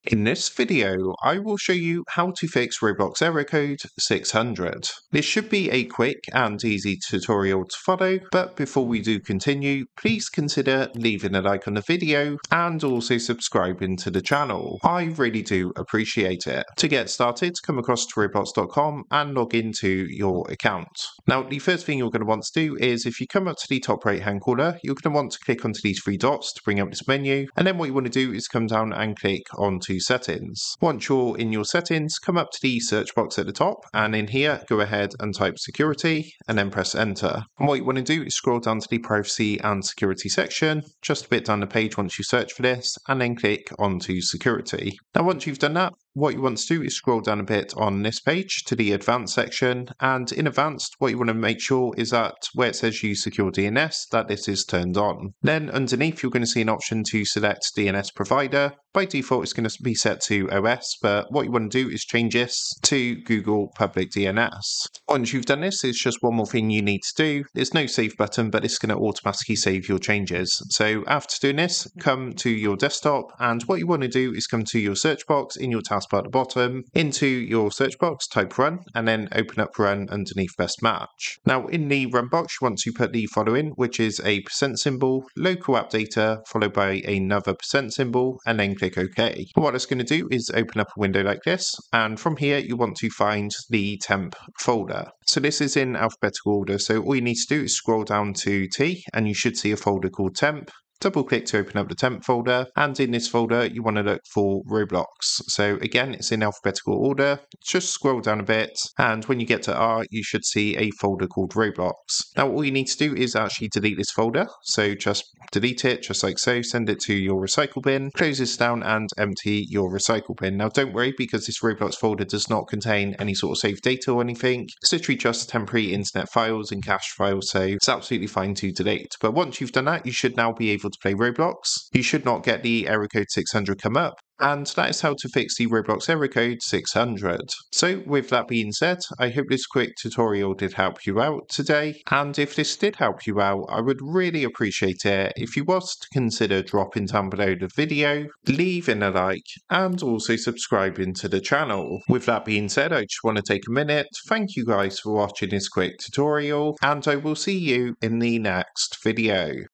How To Fix Roblox Error Code 600
Struggling with Roblox Error Code 600? This easy, step-by-step guide will help you fix Roblox Error Code 600 fast so you can get back to playing your favorite games! Discover proven solutions for connection failed errors, server connection problems, and issues joining games on Windows 10, Windows 11, Mac, and mobile devices. Learn how to troubleshoot Roblox connectivity, clear cache, reset network settings, and resolve common Roblox errors that block your gameplay. Whether you’re a beginner or an experienced player, these fixes cover Roblox login problems, server issues, and game loading errors. Follow along and solve Error Code 600 in minutes!

Thanks!